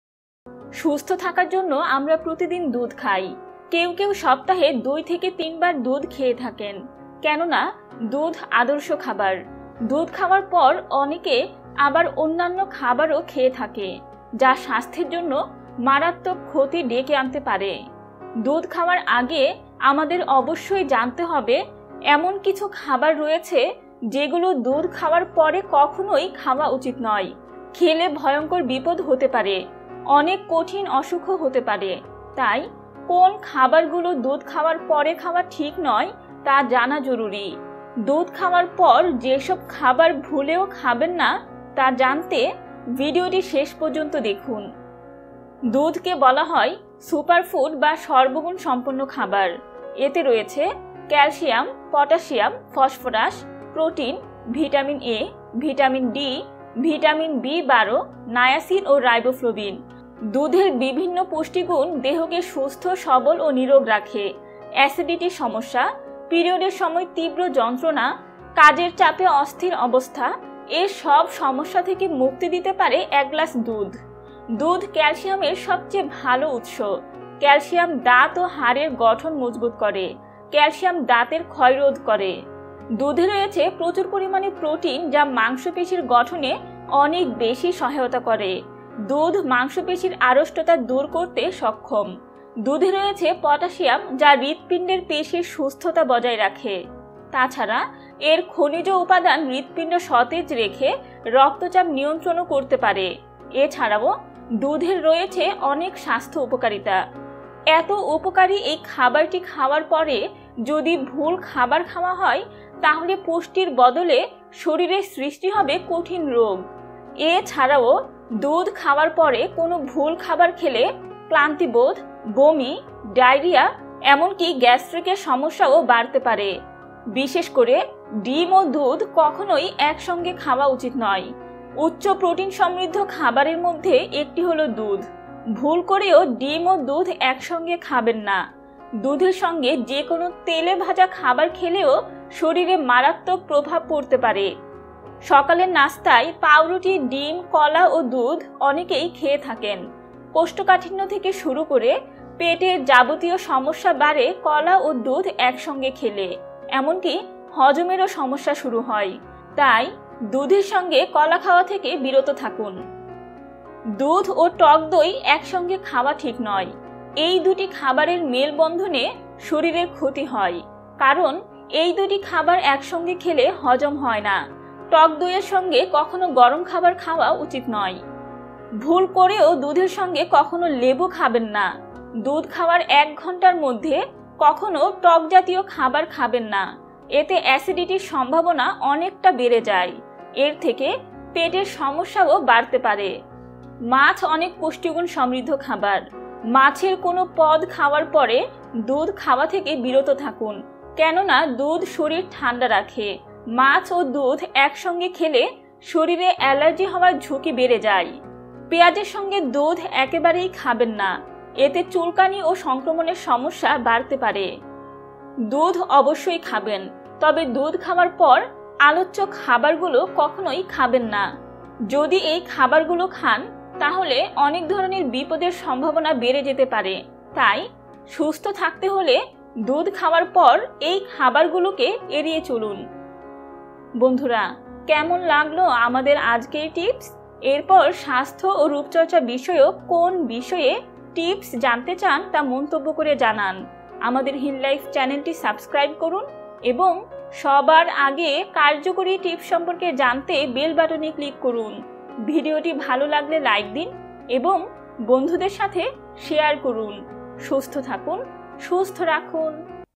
দুধ খাওয়ার আগে ও পরে এই ১০ টি খাবার ভুলেও খাবেন না | খেলেই শেষ | Milk | Health Tips
দুধের সঙ্গে যে খাবারগুলো খাওয়া বিপজ্জনক | দুধের সঙ্গে ভুলেও যেসব খাবার খাবেন না | দুধ খাওয়ার আগে ও পরে এই ১০ টি খাবার ভুলেও খাবেন না

সুস্থ থাকার জন্য আমরা প্রতিদিন দুধ খাই। কেননা দুধ আদর্শ খাবার। প্রতিদিন দুধ খাওয়ার উপকারিতা অনেক। দুধ খাওয়ার সঠিক সময় ও সঠিক নিয়ম মেনে দুধ খেলে অনেক উপকার পাওয়া যায়। মাংসপেশির আড়ষ্টতা দূর হয়। শরীরের ক্লান্তি দূর হয়। দুধের পুষ্টিগুণ ও উপকারিতার জন্যই দুধ খেলে শরীর কর্মক্ষম হয়। তবে দুধ খাওয়ার সঙ্গে কিছু ভুল খাবার খাওয়া উচিত নয়। খেলে স্থ্যের জন্য মারাত্মক ক্ষতি হতে পারে।  দুধ খাওয়ার আগে আমাদের অবশ্যই জানতে হবে, এমন কিছু খাবার রয়েছে যেগুলো দুধ খাওয়ার পরে কখনোই খাওয়া উচিত নয়। খেলে ভয়ঙ্কর বিপদ হতে পারে। অনেক কঠিন অসুখ হতে পারে। তাই কোন খাবারগুলো দুধ খাওয়ার পরে খাওয়া উচিত নয় তা জানা জরুরি। দুধ খাওয়ার পর যেসব খাবার ভুলেও খাবেন না তা জানতে ভিডিওটি শেষ পর্যন্ত দেখুন।

সুস্থ থাকুন, সুস্থ রাখুন!